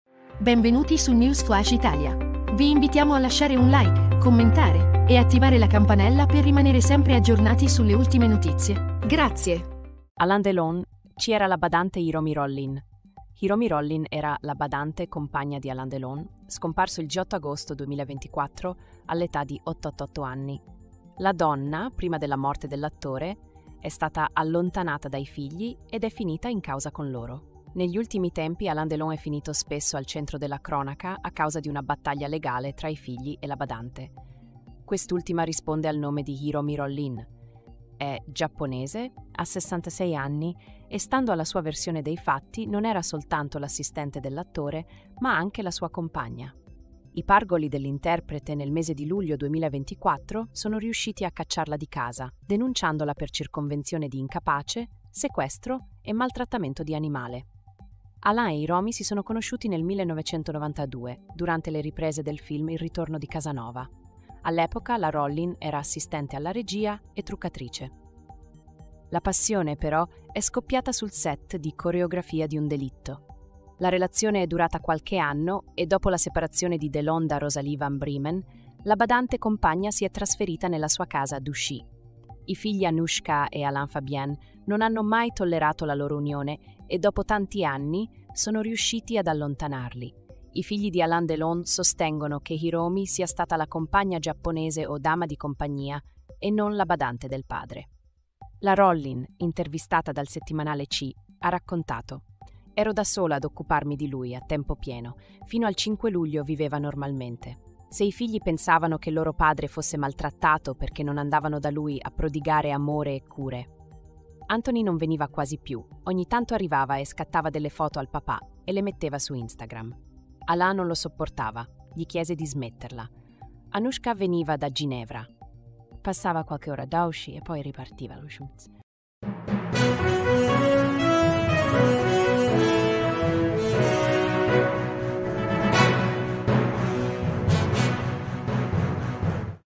ALAIN DELON: LA BADANTE GIAPPONESE ALLONTANATA DAI FIGLI INGIUSTAMENTE
Alain Delon, chi era la badante Hiromi Rollin

Hiromi Rollin era la badante/compagna di Alain Delon, scomparso il 18 agosto 2024 all'età di 88 anni. La donna, prima della morte dell'attore, è stata allontanata dai figli ed è finita in causa con loro.

Negli ultimi tempi, Alain Delon è finito spesso al centro della cronaca a causa di una battaglia legale tra i figli e la badante. Quest'ultima risponde al nome di Hiromi Rollin, è giapponese, ha 66 anni e, stando alla sua versione dei fatti, non era soltanto l'assistente dell'attore ma anche la sua compagna. I pargoli dell'interprete, nel mese di luglio 2024, sono riusciti a cacciarla di casa, denunciandola per circonvenzione di incapace, sequestro e maltrattamento di animale.

Alain e Hiromi si sono conosciuti nel 1992, durante le riprese del film "Il ritorno di Casanova". All'epoca, la Rollin era assistente alla regia e truccatrice. La passione, però, è scoppiata sul set di "Coreografia di un delitto". La relazione è durata qualche anno e, dopo la separazione di Delon da Rosalie van Breemen, la badante/compagna si è trasferita nella sua casa a Douchy. I figli Anouchka e Alain Fabien non hanno mai tollerato la loro unione e, dopo tanti anni, sono riusciti ad allontanarli.

I figli di Alain Delon sostengono che Hiromi sia stata la compagna giapponese o dama di compagnia e non la badante del padre. La Rollin, intervistata dal settimanale Chi, ha raccontato: "Ero da sola ad occuparmi di lui, a tempo pieno. Fino al 5 luglio viveva normalmente. Se i figli pensavano che loro padre fosse maltrattato perché non andavano da lui a prodigare amore e cure? Anthony non veniva quasi più, ogni tanto arrivava e scattava delle foto al papà e le metteva su Instagram. Alain non lo sopportava, gli chiese di smetterla. Anouchka veniva da Ginevra, passava qualche ora a Douchy e poi ripartiva. Lui mi diceva 'Ti sposerò prima di morire'".